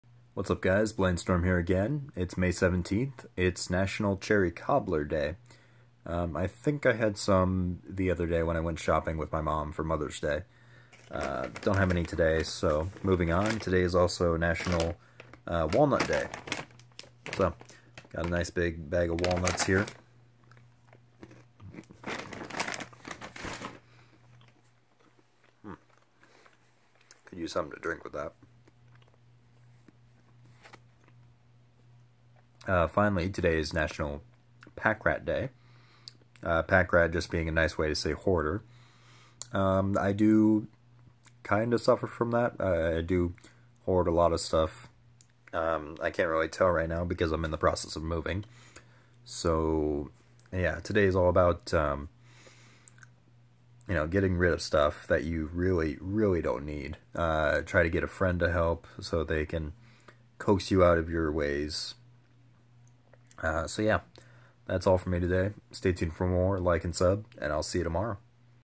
Storm 365 - May 17: National Pack Rat Day
I'm a sheep herder. It's pronounced hoarder and yes you are. Celebrating National Cherry Cobbler Day, Walnut Day, and Pack Rat Day!